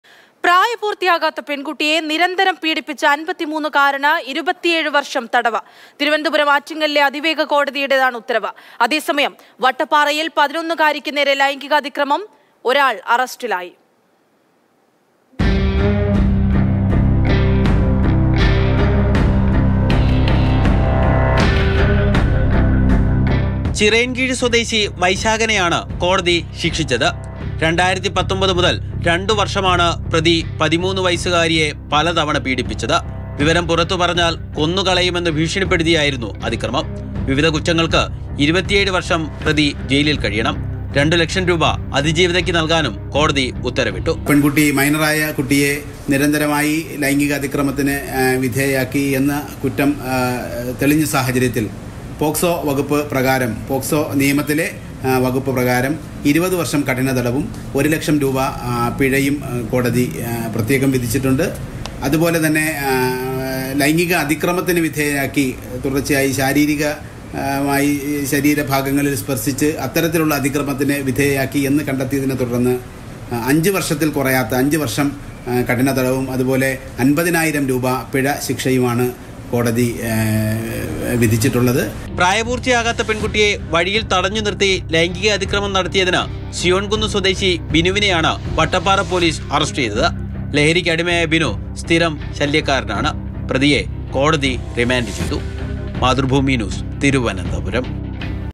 പ്രായപൂർത്തിയാകാത്ത പെൺകുട്ടിയെ തുടരെ പീഡിപ്പിച്ച 53 കാരന് 27 വർഷം തടവ് | Thiruvanathapuram
പ്രായപൂർത്തിയാകാത്ത പെൺകുട്ടിയെ തുടരെ പീഡിപ്പിച്ച 53 കാരന് 27 വർഷം തടവ് | Thiruvanathapuram | Crime |

Happy viewing!